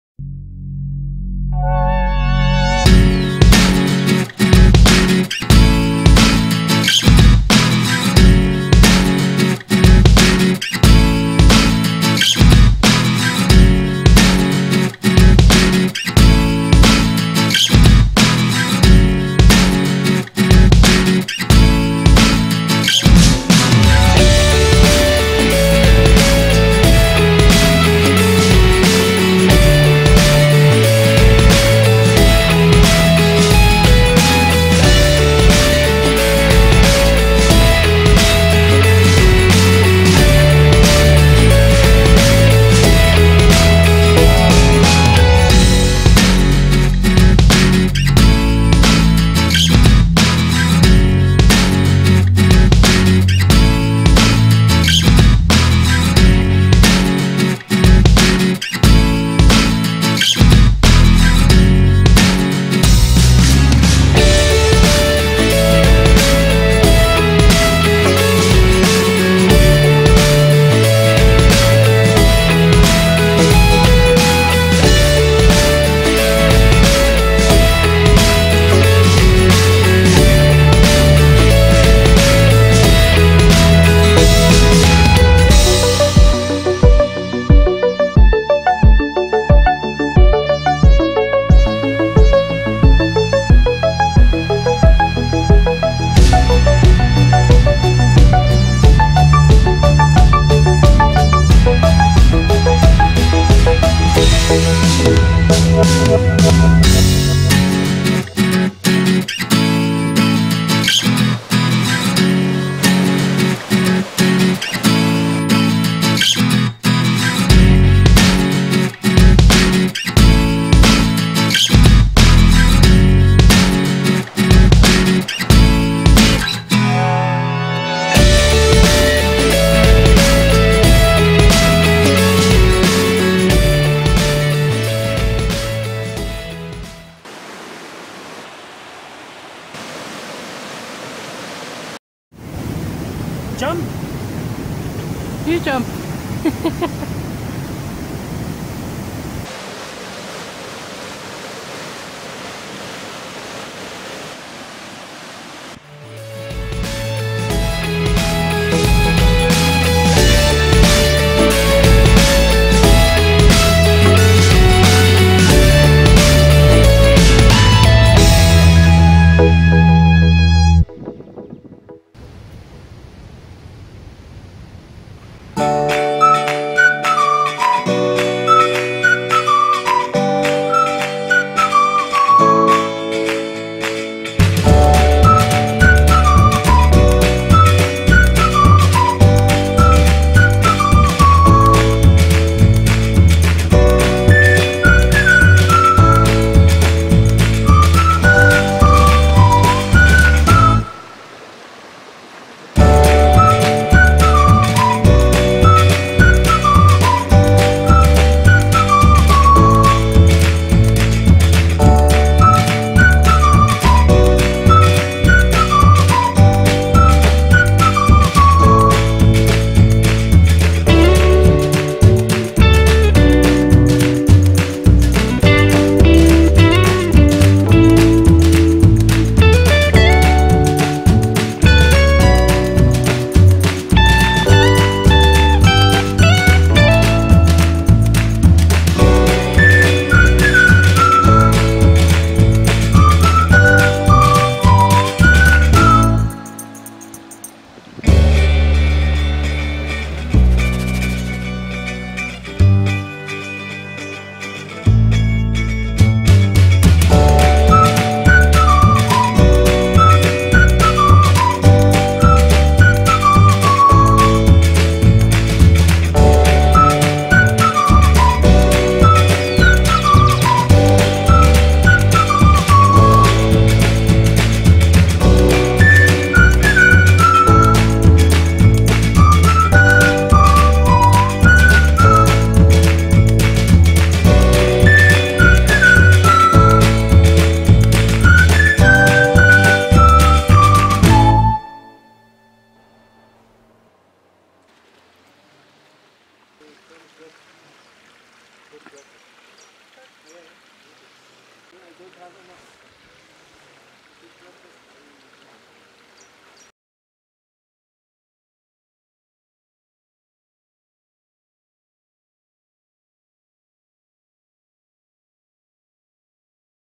Papallacta Hot Springs/Termas Papallacta-Ecuador
A trip to Termas Papallacta hot springs to relax, pamper yourself and enjoy. Papallacta, the doorway to the Amazonas

Un viaje a Termas Papallacta para relajarse, mimarse y disfrutar, Papallacta, la entrada al Amazonas